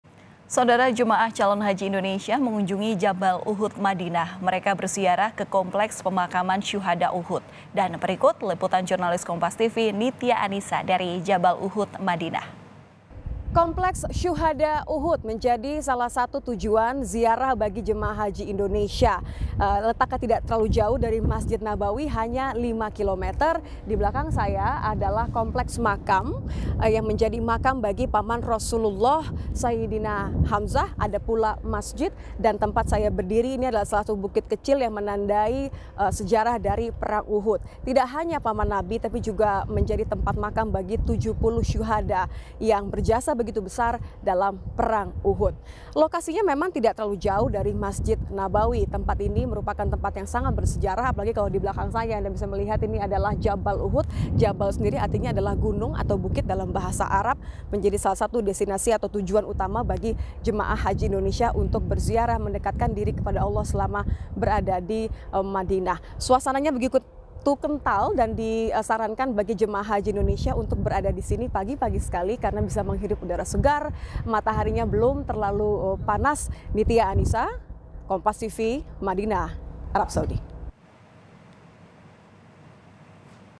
Jemaah Haji Indonesia Kunjungi Jabal Uhud, Telurusi Sejarah Perjuangan Sahabat Nabi
MADINAH, KOMPAS.TV - Jemaah calon haji Indonesia mengunjungi Jabal Uhud, Madinah.

Mereka berziarah ke Kompleks Permakaman Syuhada Uhud.

Komplek syuhada di Jabal Uhud menjadi salah satu tempat yang wajib dikunjungi oleh Jemaah haji 2022.

Di sini, umat Islam bisa menapaki perjuangan para sahabat Nabi Muhammad.

Sebagaimana dilaporkan jurnalis Kompas TV Nitia Annisa dari Jabal Uhud, Madinah, para Jemaah haji 2022 pada hari Kamis waktu setempat tampak menaiki jabal uhud atau gunung Uhud untuk berziarah.

Baca Juga Setelah 11 Tahun Menanti, Jemaah Haji Sujud Syukur saat Tiba di Madinah di

Nitia Annisa juga menunjuk tempat ia berdiri saat ini, yakni sebuah bukit kecil sebagai penanda perang Uhud.

Jaraknya tak jauh dari lokasi pemakanan masjid syuhada yang berada di antara jabal Uhud.

Dari masjid Nabawi, lanjut Nitia Annisa, jabal uhud sendiri jaraknya kurang lebih 5 kilomoter dan jadi destinasi utama para peziarah haji 2022 untuk menapai perjuangan Nabi dan para sahabat.